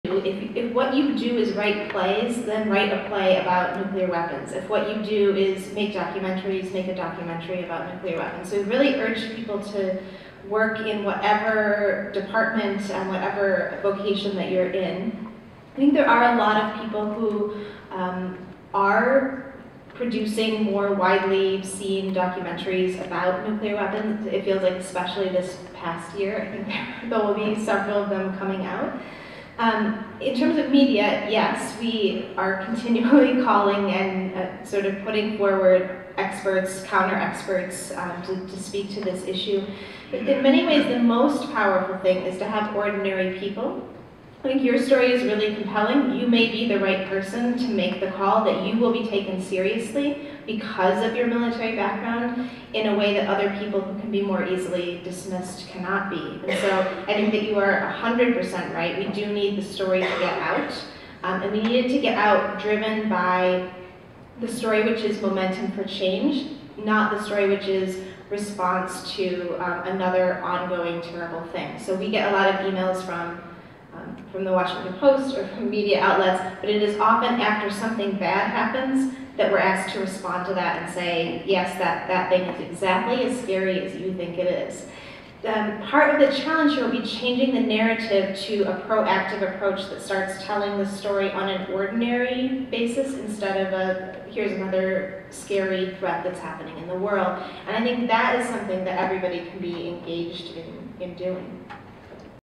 Being Proactive (Drs. Bolton & Welty of ICAN #10)
"If what you do is write plays, then write a play about nuclear weapons. If what you do is write documentaries. Really encourage people to work in whatever departments and whatever vocation you are in. There are a lot of people who are widely seeing more documentaries about nuclear weapons, especially this past year. The part of the challenge will be changing the narrative to a proactive approach that starts telling the story on its ordinary basis instead of hearing another scary threat that’s happening."

Two speakers from the International Campaign to Abolish Nuclear Weapons, Matthew Bolton, PhD, and Emily Welty, PhD, spoke April 28, 2018 at Community of Christ Stone Church in Independence, Missouri. Dr. Bolton is associate chair of political science at Pace University in New York and a former resident of Independence, MO. Dr. Welty is vice moderator of the World Council of Churches Commission on International Affairs. Their talk emphasized a humanitarian approach to the international concern about nuclear weapons.

Video by Mark and Jenny Semet